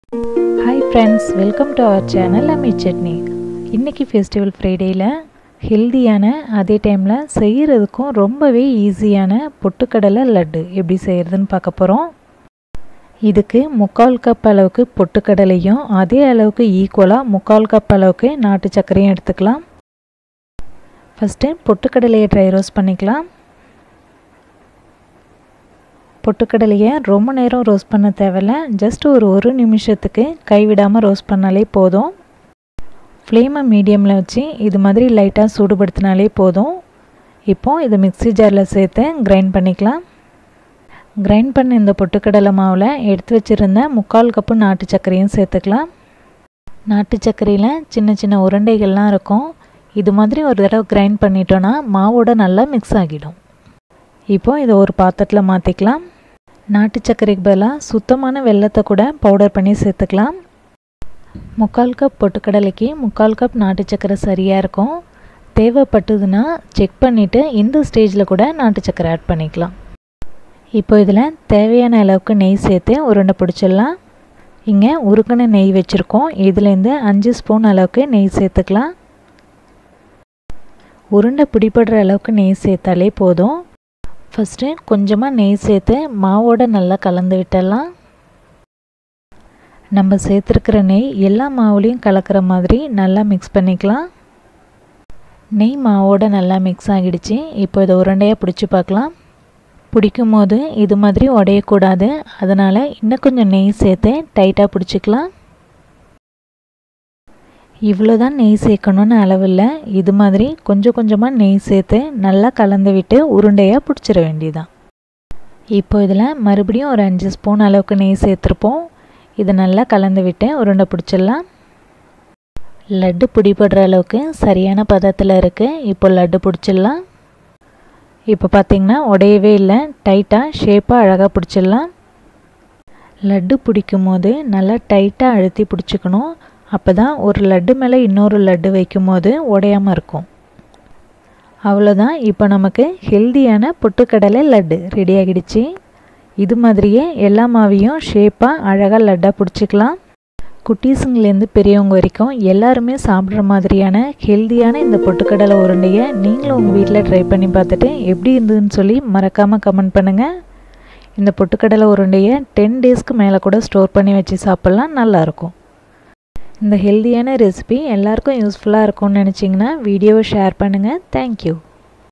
பொட்டுக்கடலை உருண்டை/ pottukadalai urundai recipe/ pottukadalai laddu/jaggery sweet recipes in tamil
This video shows how to make pottukadalai urundai / pottukadalai laddu, an easy jaggery sweet recipe. To make this, we need just three ingredients.

roasted gram - 3/4 cup
jaggery powder - 3/4 cup
ghee

METHOD:
Dry roast the roasted gram for a minute in medium flame.
Grind it once, then add jaggery powder.
Grind it again and then transfer to a mixing bowl.
Add ghee gradually & mix it well.
Make even size balls.

Now our tasty pottukadalai urundai / pottukadalai laddu is ready.

Friends, make this pottukadalai urundai / pottukadalai laddu for your kids... For sure, everybody will like it